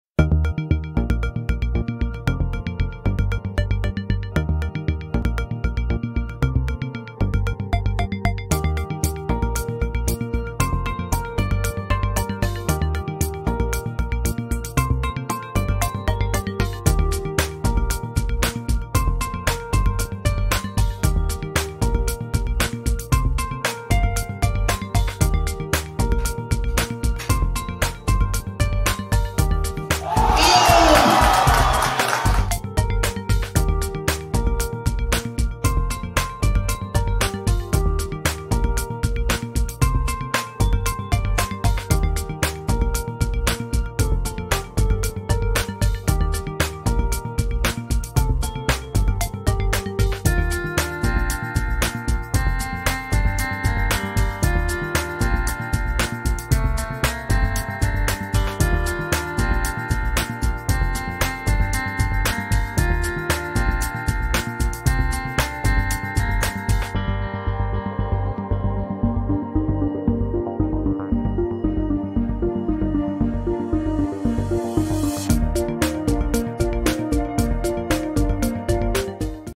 مصري يحقق أعلى قفزة من الماء!
مصر تسجل رقما قياسيا في موسوعة جينيس للأرقام القياسية لأعلى قفزة من الماء للسباح عمر شعبان! 🏊🏻🇪🇬